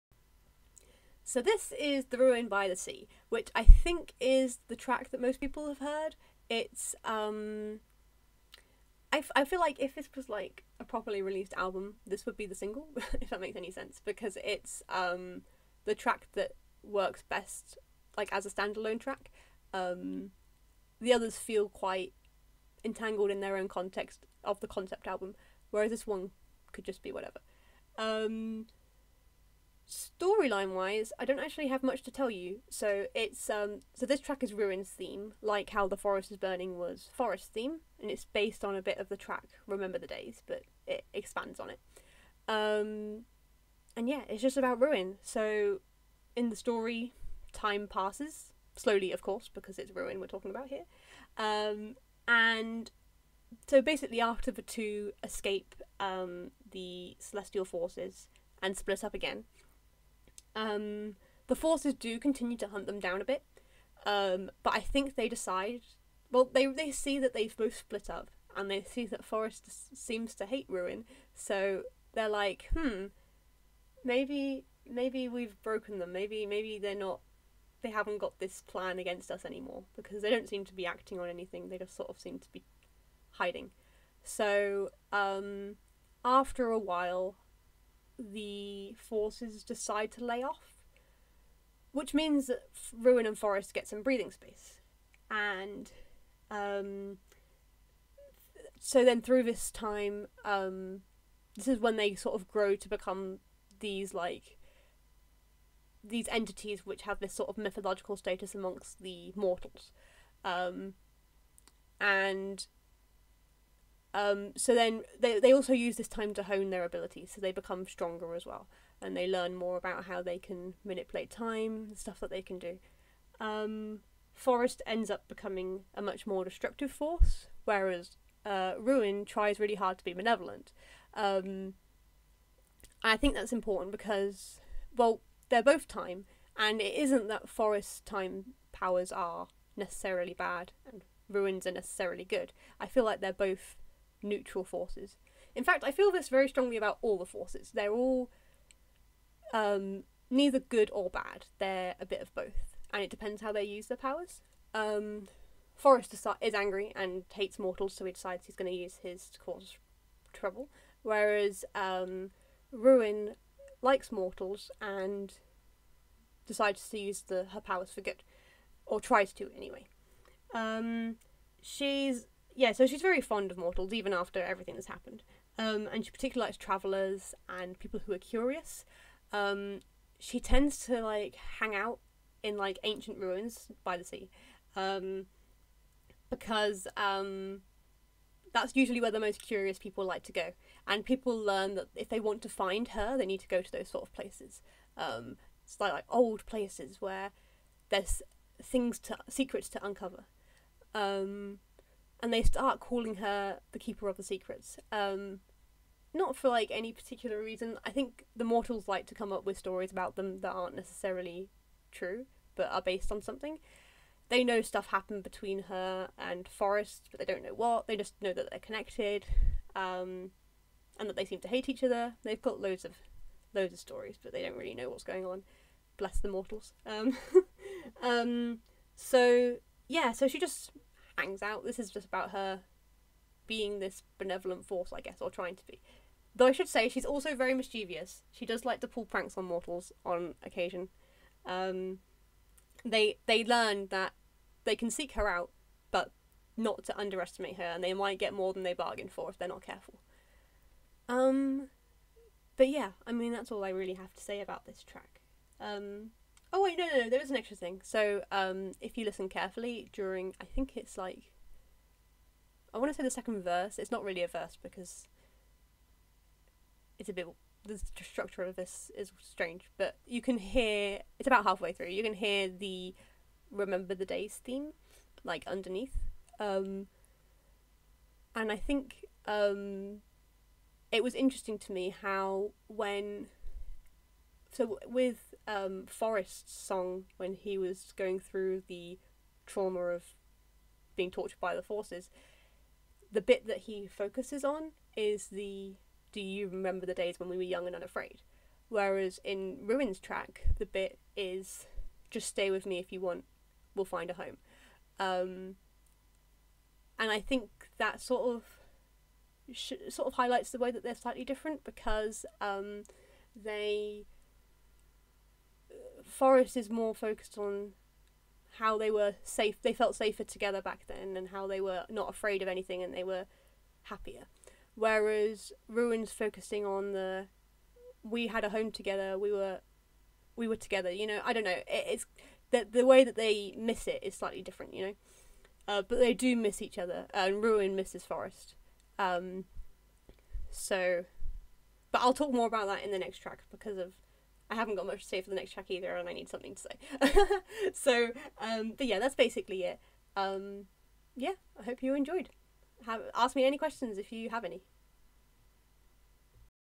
6. OUTT Commentary - The Ruin By The Sea
TRANSCRIPT AVALIABLE

Apparently the transcript is too long to fit into the description box, alas, so I'm going to throw all of the transcripts onto google docs, and I'll add the link to the google drive folder in here so you guys can access it if you want!

This link should work, but if it doesn't, yell at me and I'll fix it: